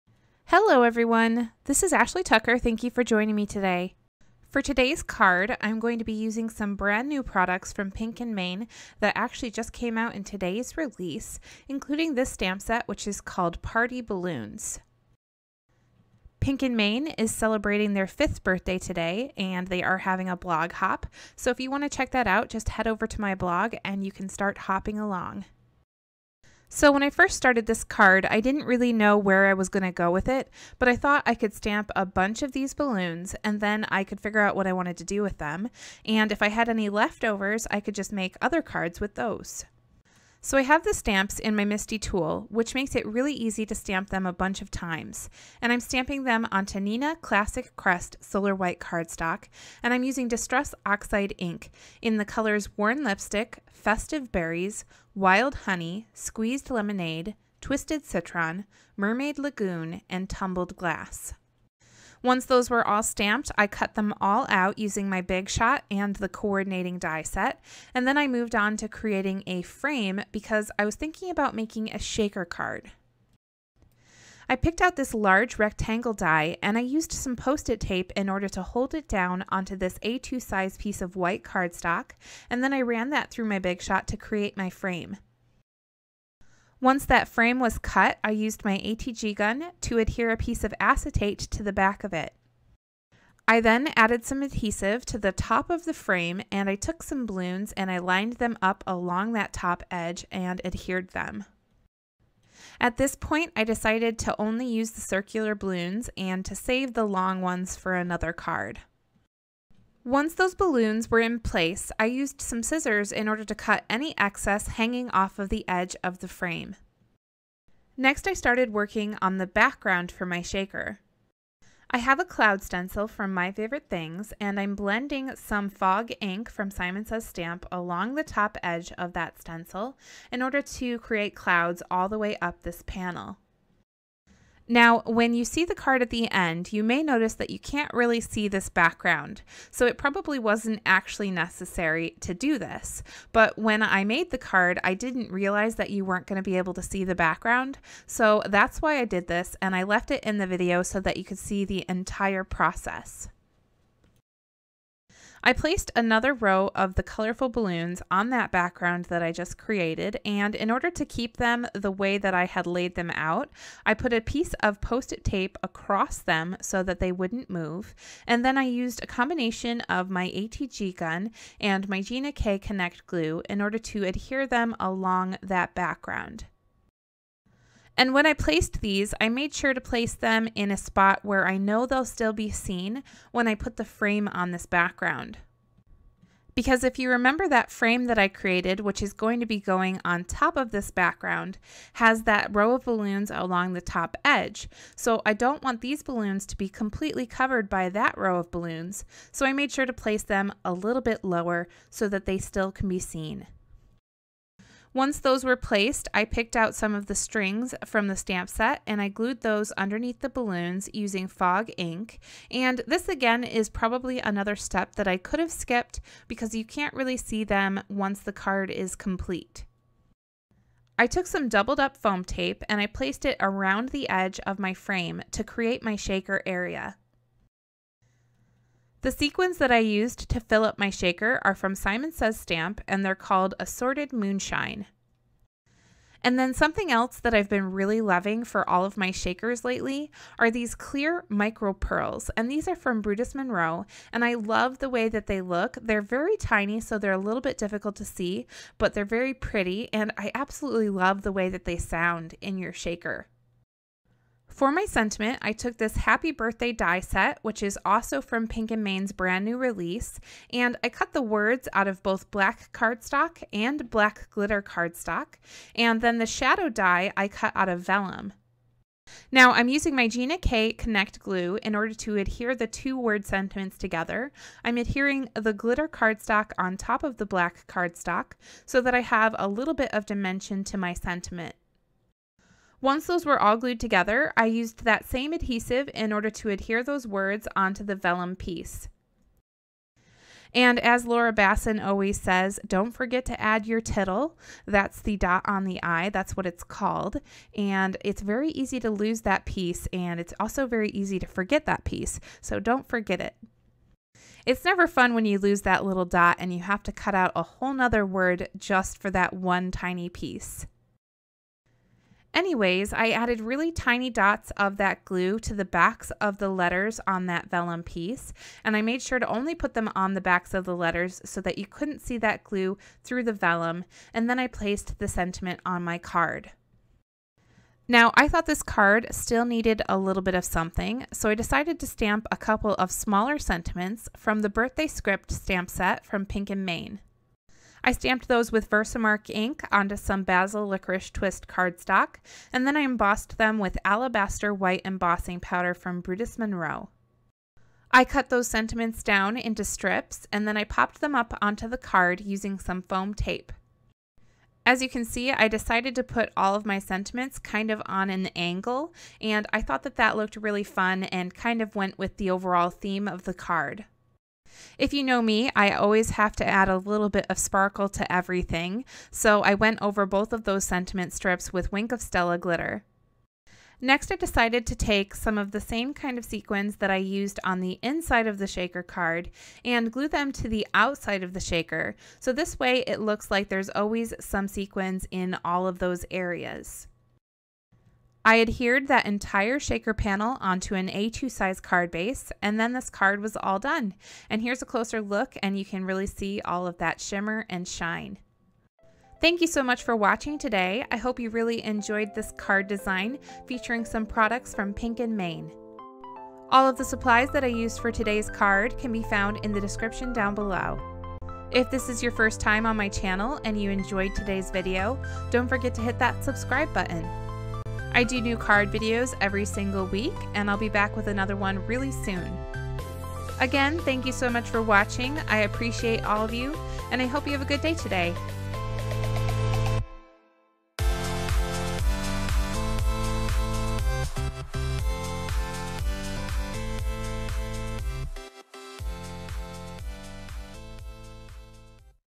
Birthday Balloon Shaker Card | Pink & Main's 5th Birthday Blog Hop!
Hello my friends! Today I am celebrating with Pink and Main for their 5th birthday and we are partying with a blog hop!

• Post-it Tape

*Products marked with an asterisk have been provided by the company. All other products were purchased by me.